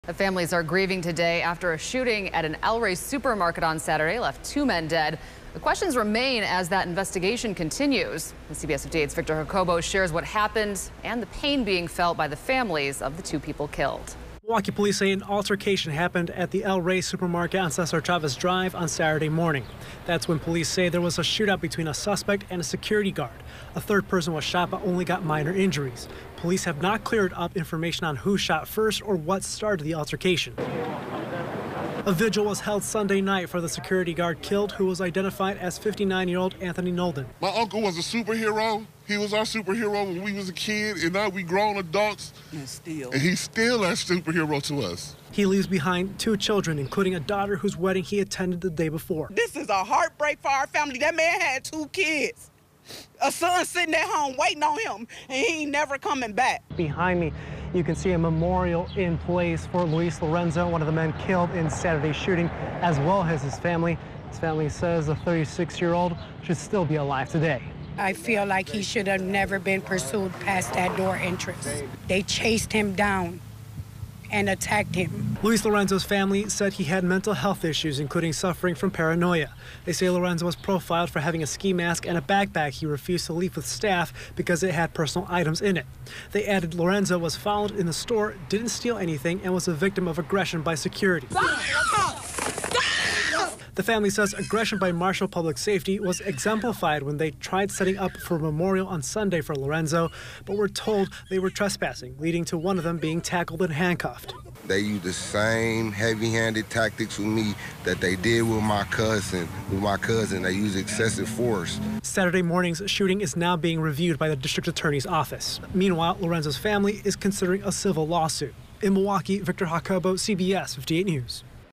Families of men killed at El Rey shooting left with pain, unanswered questions
Two families are mourning and a south side community left in shock after a shooting at El Rey supermarket left two men dead and a third bystander injured.

Milwaukee police said the shooting happened Saturday, July 9 at the El Rey supermarket on South Cesar Chavez Drive in the morning.